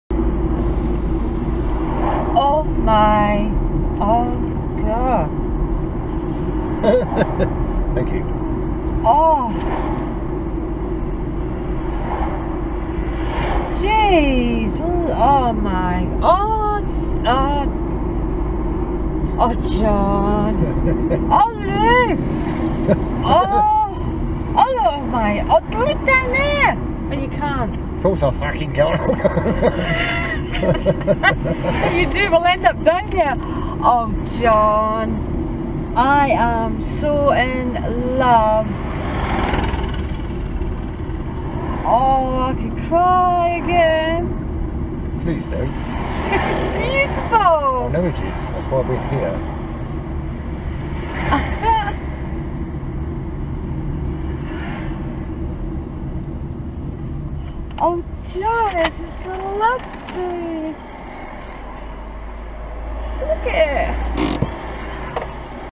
First view of the coast
The reaction of someone seeing a foreign coastline for the very first time (in this case Sorrento).  Absolutely priceless